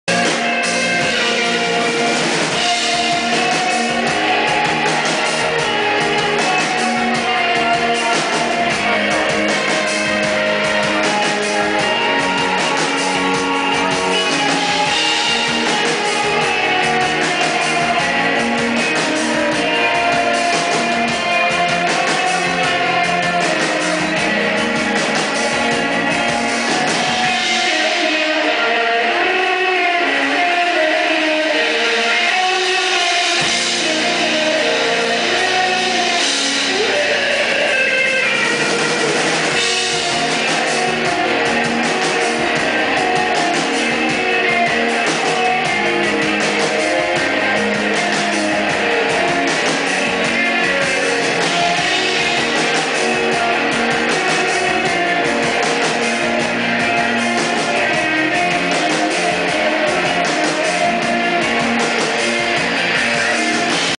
SLEEP PARTY PEOPLE at Pod Minogą (live: 25.11.2014 Poznań)
Sleep Party People played their concert at Pod Minogą in Poznań (PL) on 25. November 2014 and it was a magical night.

Read more about the show and find the photos on Good because Danish